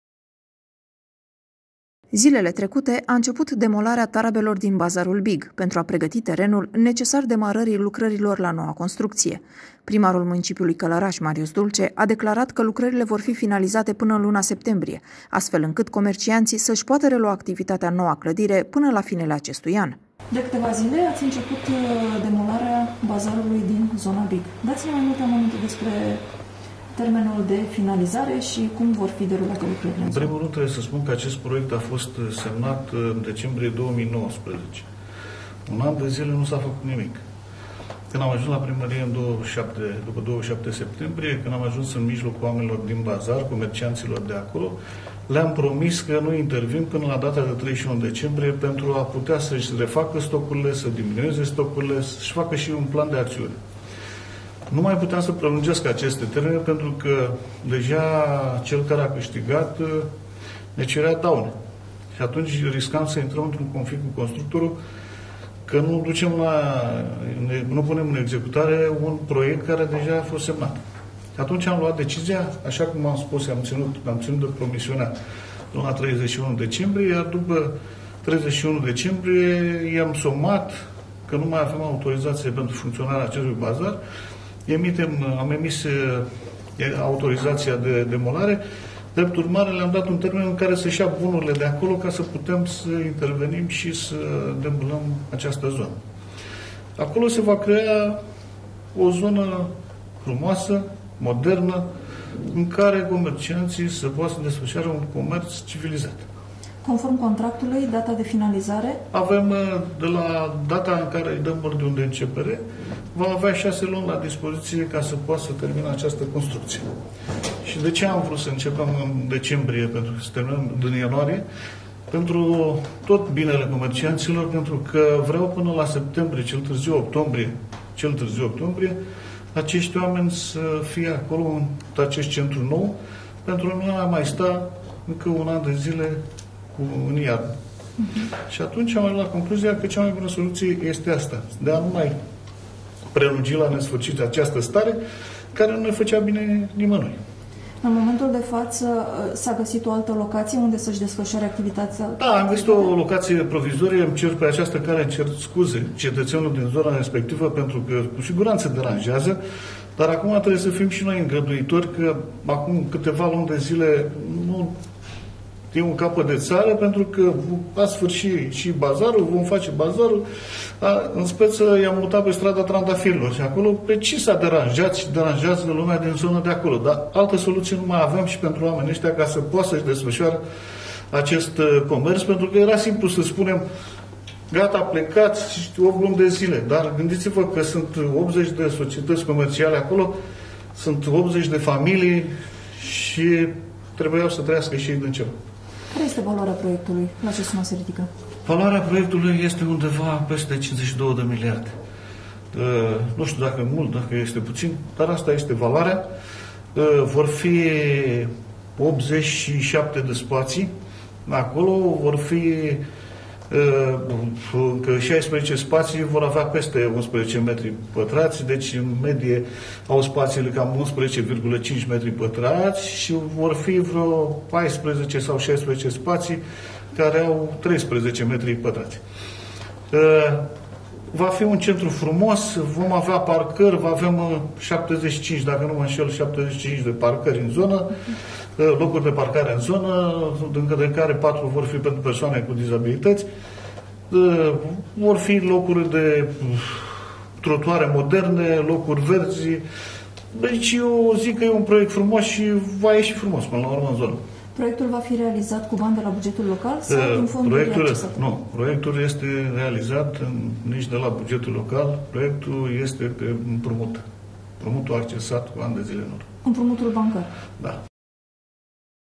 Au început lucrările la Bazar Big
Primarul Municipiului Călărași, Marius Dulce a declarat că lucrările vor fi finalizate până în luna septembrie.